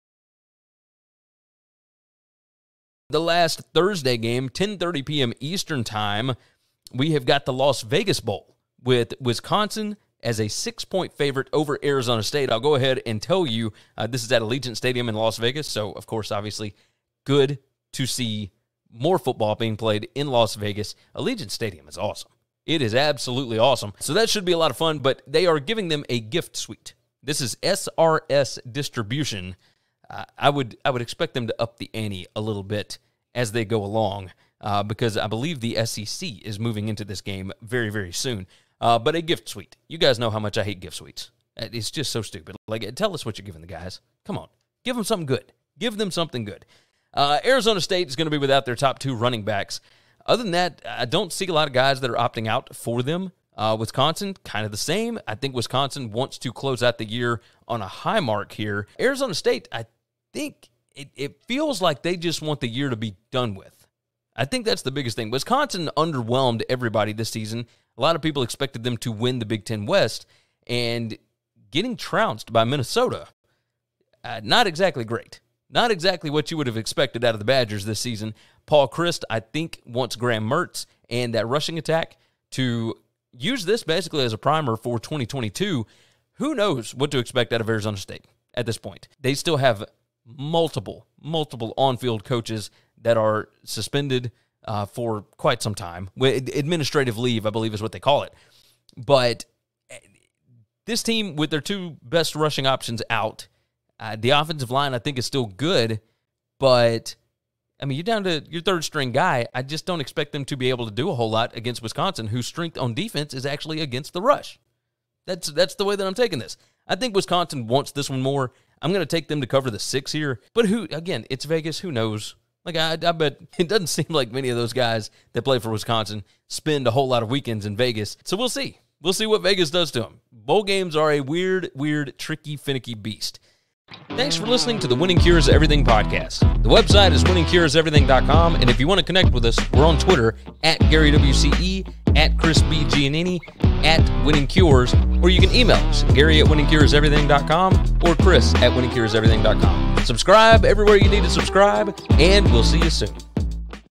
Las Vegas Bowl Wisconsin vs Arizona State Picks Against the Spread Predictions 2021 College Football
Gary & Chris give their quick preview, predictions & picks against the spread (ATS) for the college football 2021 Las Vegas Bowl featuring Wisconsin vs Arizona State!

Get a more in-depth breakdown & official picks for this game from the BetUS College Football Show Bowl Preview Pt 5